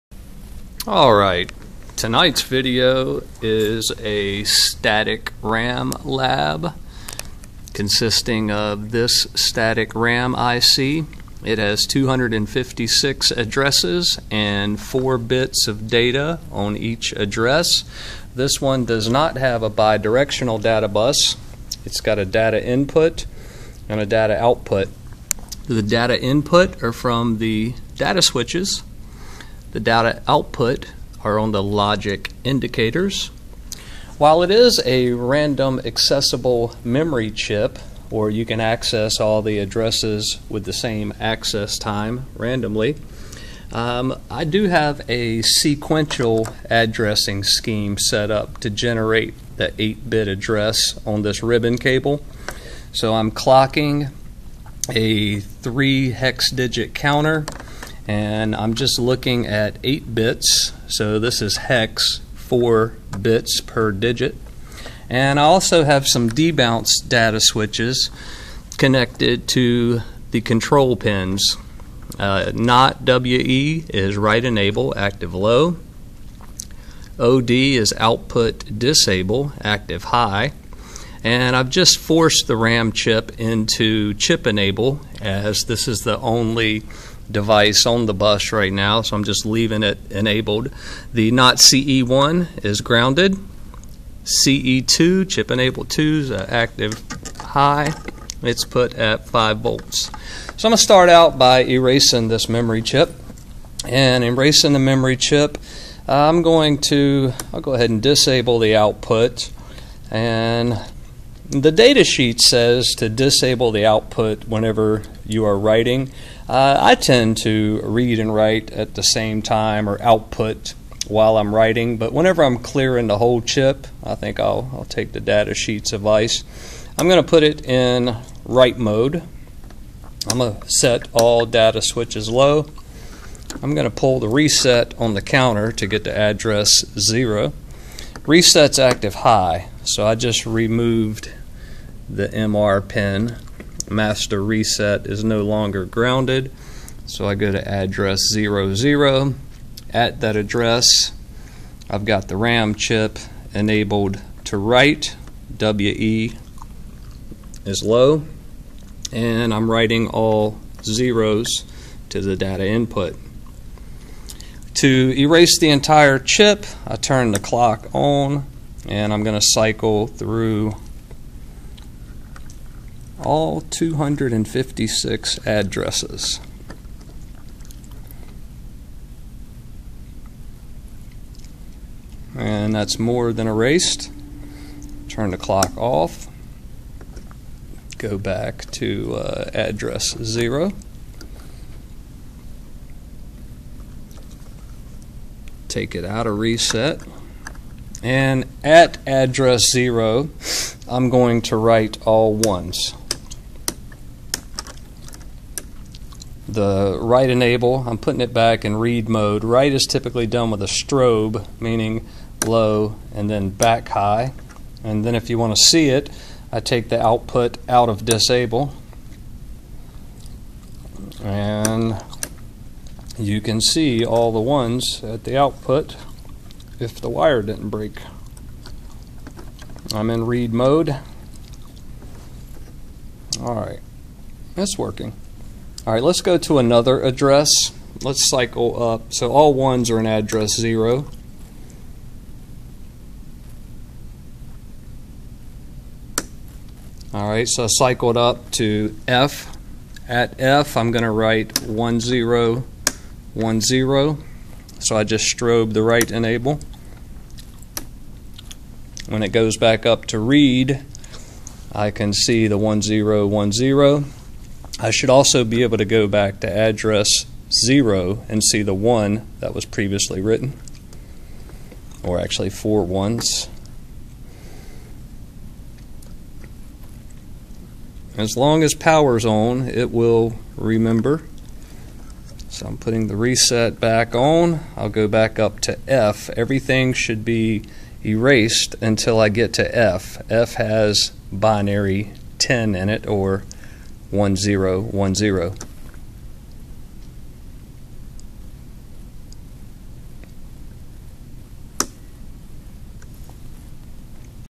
Static RAM 256x4b Address Data Control
256 x 4 configuration = 1k bit density

8 bit address bus for address 0 - 255 The HEX Range is 00 - FF
The data bus is 4 bits, and one data bus in and a separate output bus.
The control bus has four pins, two are chip enables (chip select), an output disable OD (active high disable), and write enable (WE is active low)